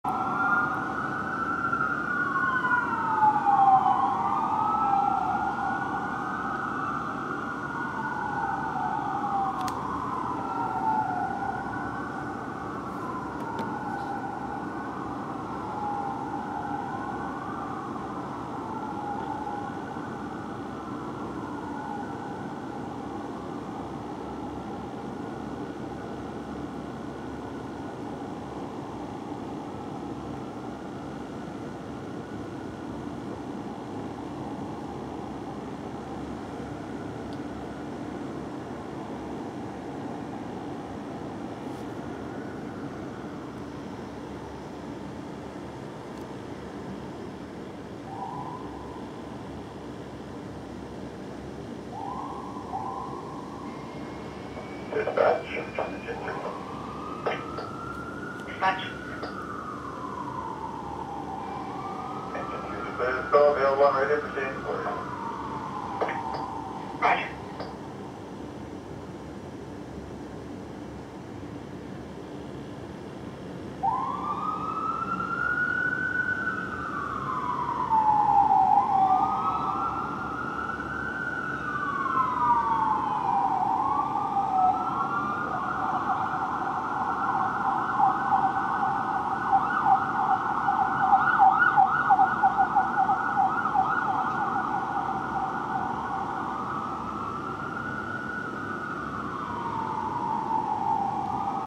Vancouver Fire Engine 7 & Ladder 7 Responding
Vancouver Fire Rescue Services Engine 7 & Ladder 7 Responding for Alarms Ringing. Sorry I was sleeping so thats why I didn't get the Engine leaving the hall